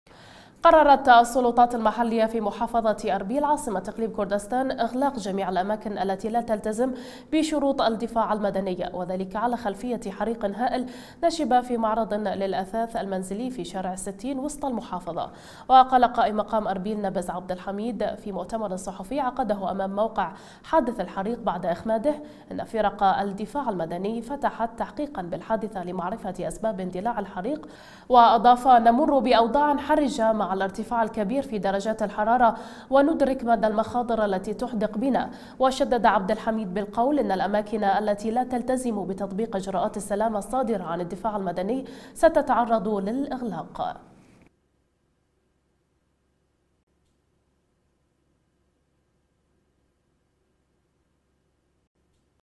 بعد حريق هائل: أربيل تقرر إغلاق الأماكن غير الملتزمة بإجراءات السلامة
بعد حريق هائل
أربيل تقرر إغلاق الأماكن غير الملتزمة بإجراءات السلامة

قررت السلطات المحلية في محافظة أربيل عاصمة إقليم كوردستان إغلاق جميع الأماكن التي لا تلتزم بشروط الدفاع المدني، وذلك على خلفية حريق هائل نشب في معرض للأثاث المنزلي في شارع الستين وسط المحافظة؛ وقال قائممقام أربيل نبز عبدالحميد في مؤتمر صحفي عقده أمام موقع حادث الحريق بعد إخماده: إن فرق الدفاع المدني فتحت تحقيقا بالحادثة لمعرف اسباب اندلاع الحريق. وأضاف، نمر بأوضاع حرجة مع الارتفاع الكبير في درجات الحرارة، وندرك مدى المخاطر التي تحدق بنا. وشدد عبدالحميد بالقول: إن الاماكن التي لا تلتزم بتطبيق اجراءات السلامة الصادرة عن الدفاع المدني ستتعرض للإغلاق.